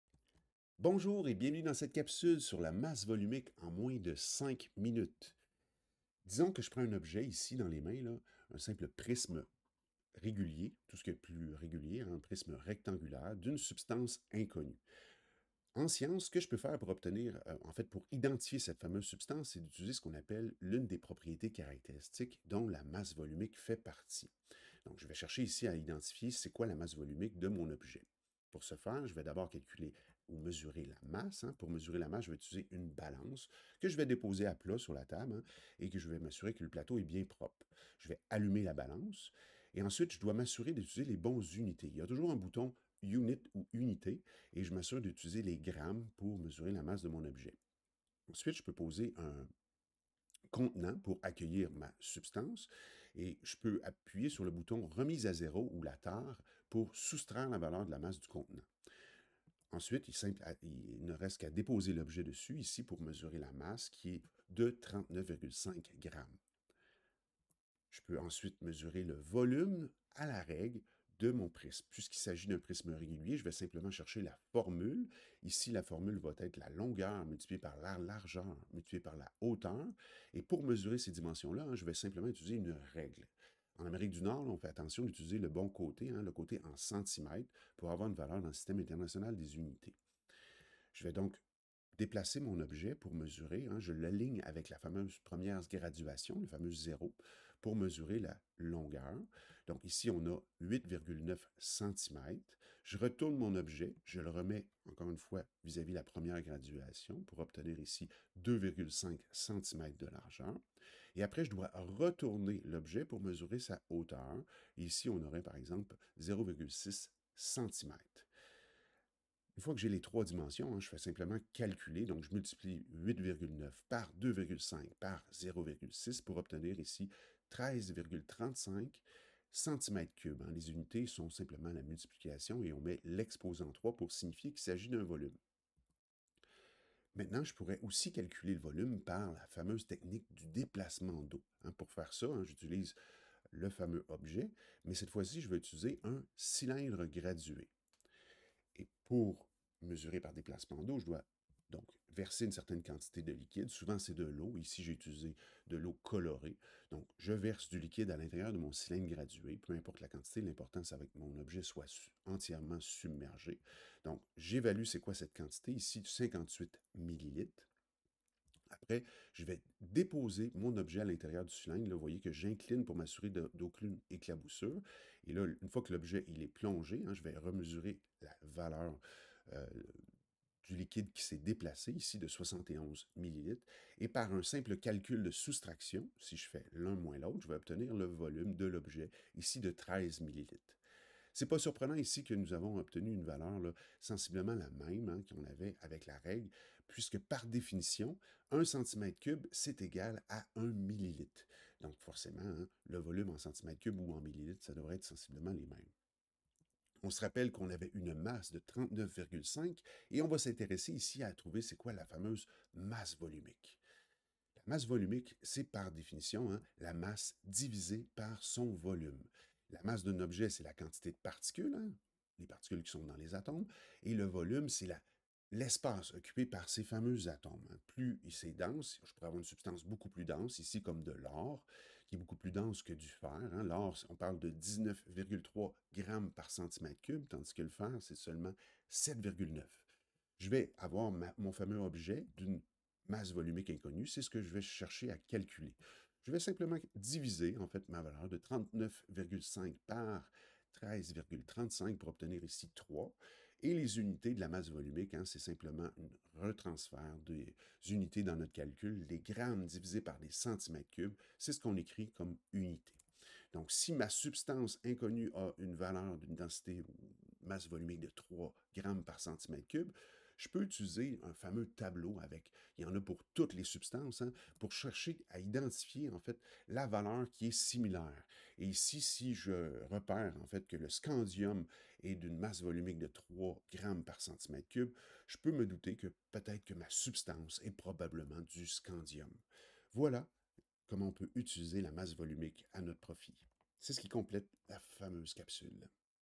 Sous-concept clé : masse volumique
L'une des propriétés caractéristiques bien utile : la masse volumique